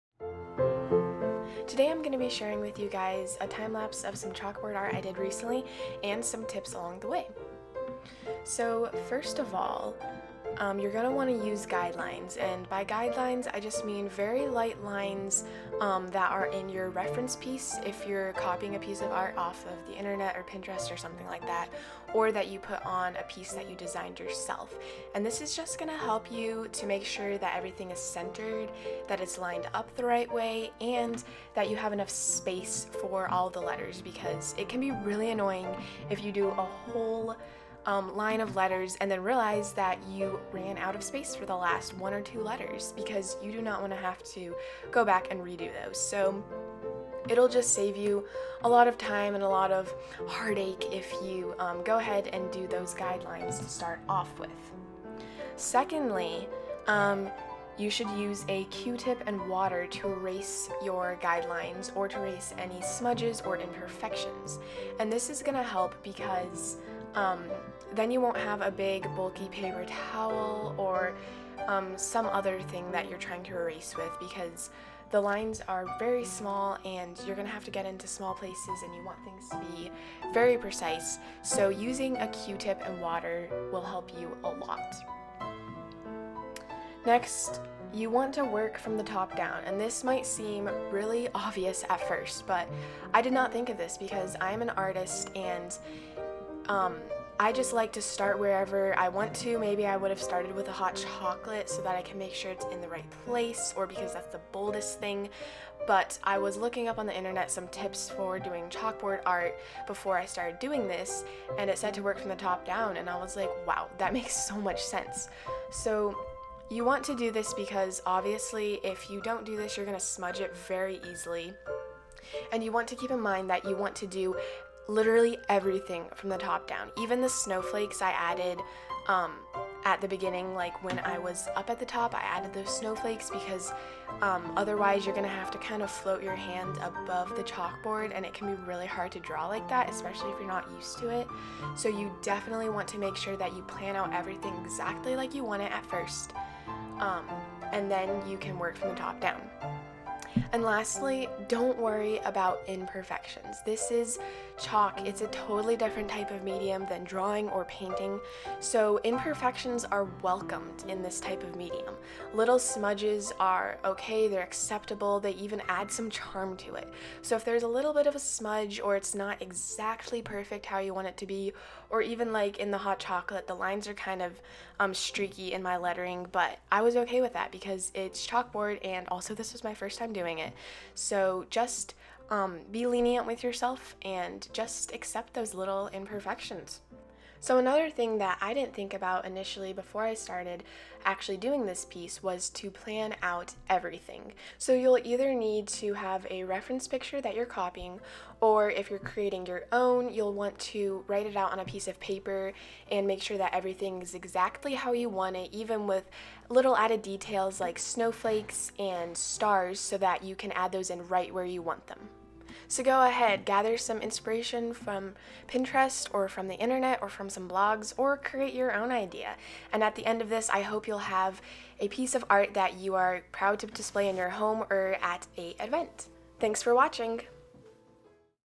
Chalkboard Art Timelapse - Hot Chocolate Station
This piece was super fun to create and film for you and I hope you enjoyed this little timelapse!

Materials Used:
Chalk Pencil
Chalk Marker